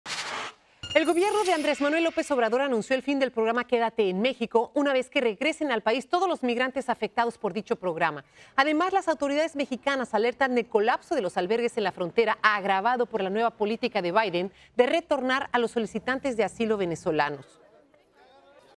AMLO anuncia que acabará con 'Quédate en México' Noticias Telemundo
Short oficial de Noticias Telemundo. El presidente hizo el anuncio mientras se alerta sobre el colapso de los albergues del lado mexicano de la frontera. Esto se agravó luego de que el Gobierno de EE.UU. implementó la medida de expulsar a los venezolanos que crucen ilegalmente al país. Disfruta del formato YouTube Shorts, videos verticales de 60 segundos para enterarte de lo más destacado en Noticias del mundo.